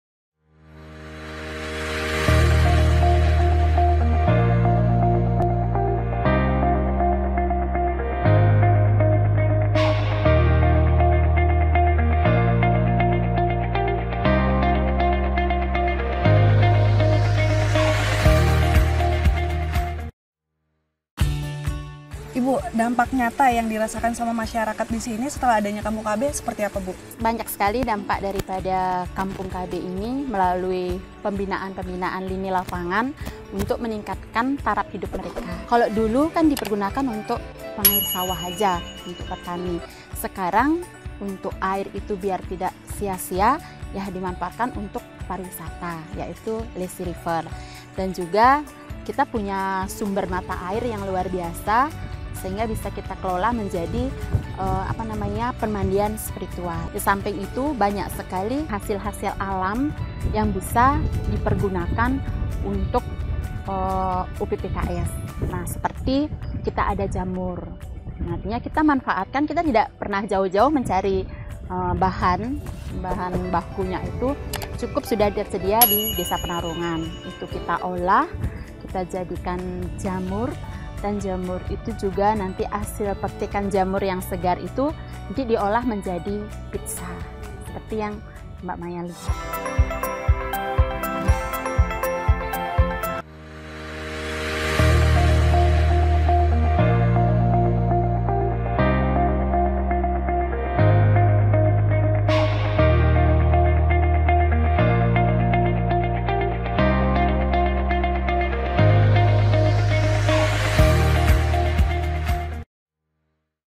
Ada Pizza Jamur di Kampung KB Penarungan | Part 2
Memanfaatkan potensi alam yang ada, kampung kb penarungan berkreasi mengoptimalkan sumber daya alam yang tersedia, seperti memanfaatkan potensi sungai menjadi potensi pariwisata, pemandiaan spritual serta mengajak masyarakat untuk budidaya jamur yang hasilnya dapat di oleh menjadi cemilan yang enak, seperti pizza jamur. simak terus yaa lanjutan perjalanan kami di kampung kb penarungan, badung-bali

Tonton terus video-video Kampung KB Kita Lainnya.

Terima Kasih
Salam Kampung KB